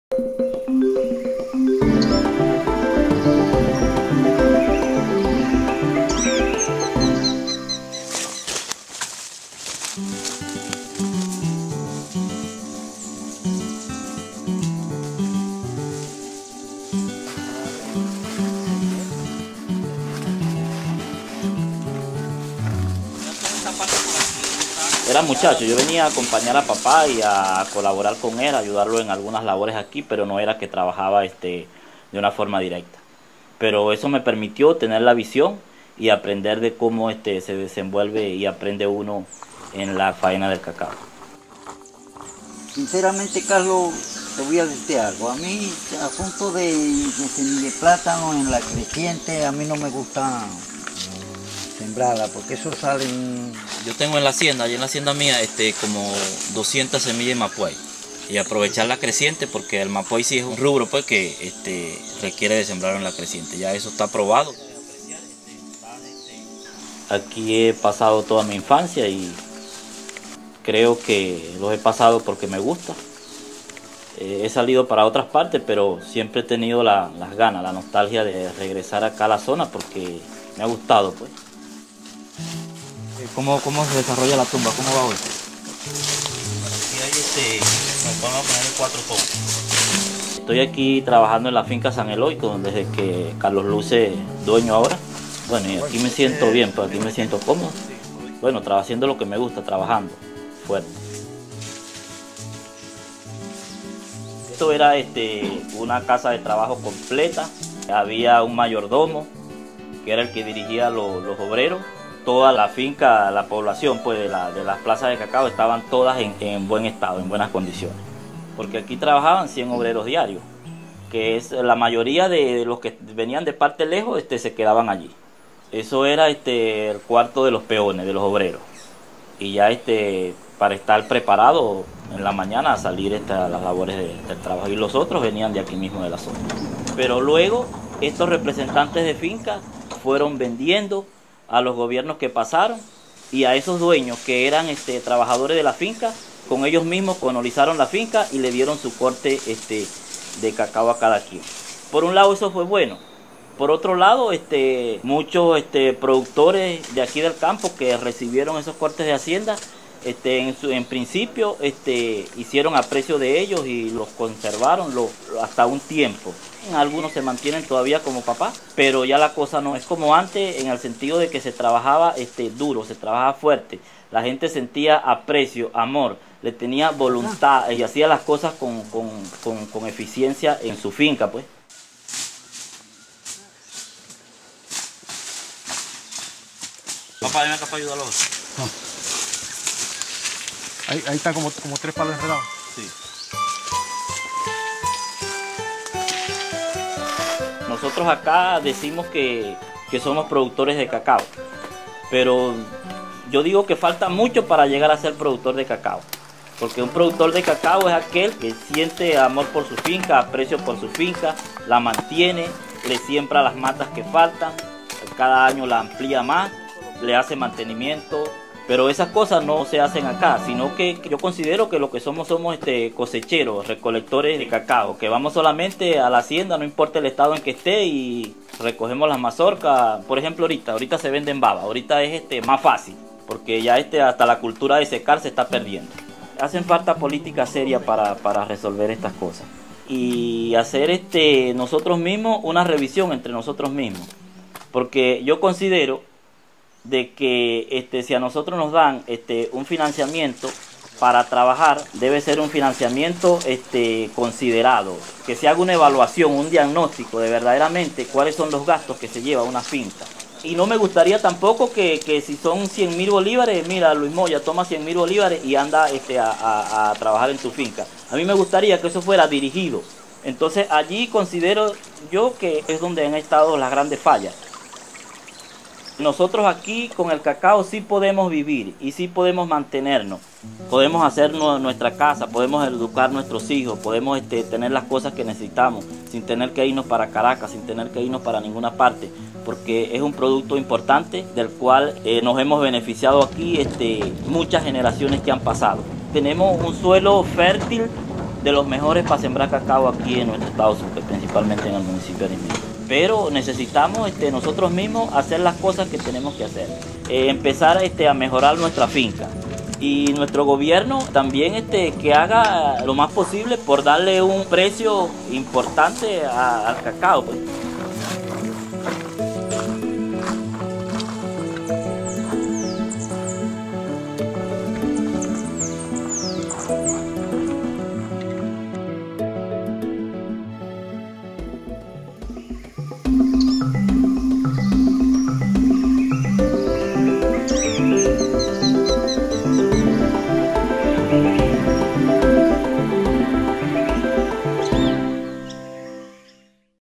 Cacao Retratos - Productores (Luis Moya) - Paria, Edo. Sucre
Luis Moya, productor de la Península de Paria, aprendió sobre siembra de cacao con su padre. Hoy trabaja para la misma hacienda en la que trabajó él hace 60 años, solo que hoy esta prácticamente en ruinas.